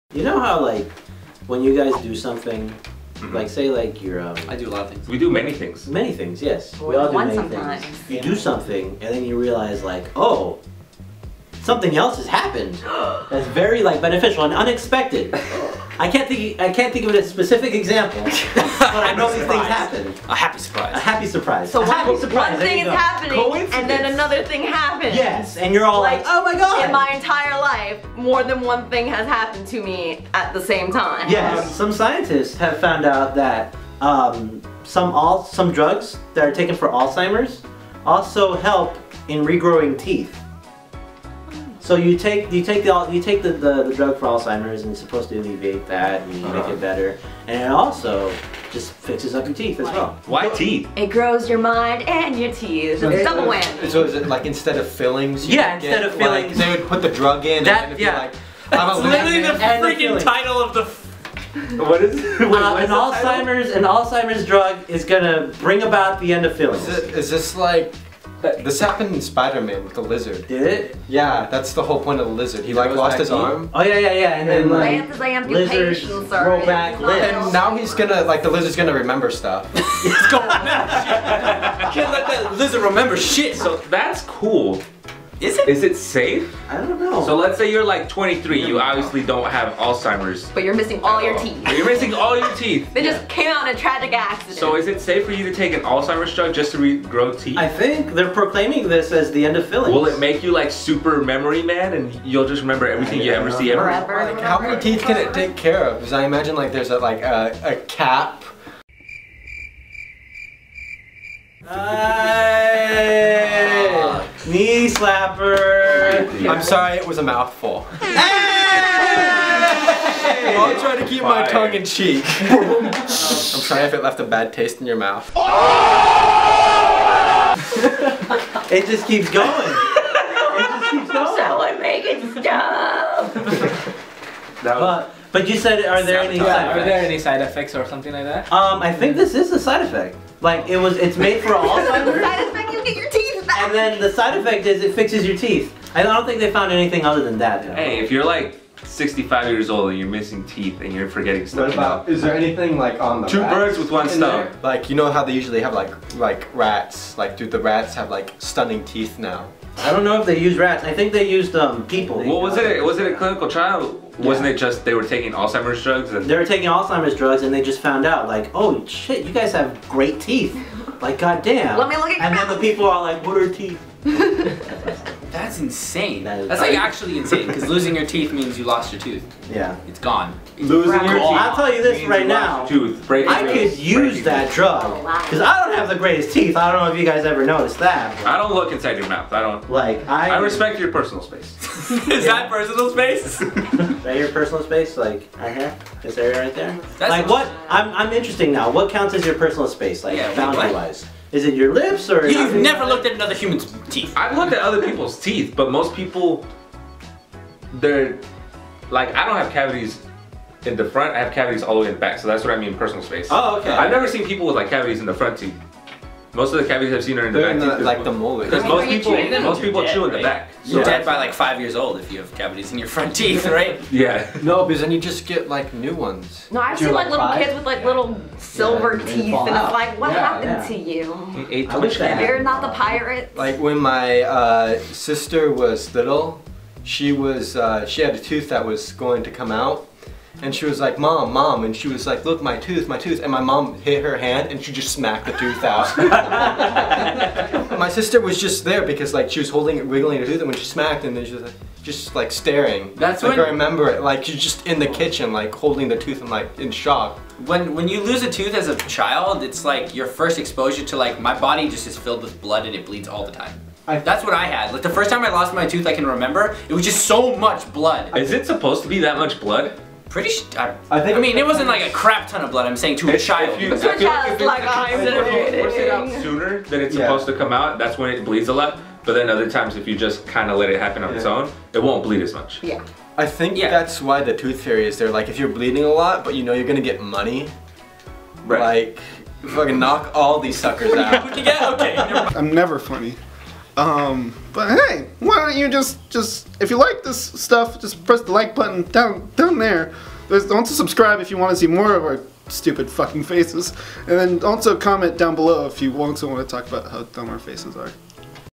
Alzheimer's Medicine Can Grow Back Teeth! - Round Trip # 13
Scientists accidentally discovered a way to grow back teeth when they tested a new drug for Alheimer's.

Hosted by: Daniel Farias
Starring: Alex Ramos, Michael Fricke, Alison Meyers, John Muller, Harvey Karuppusamy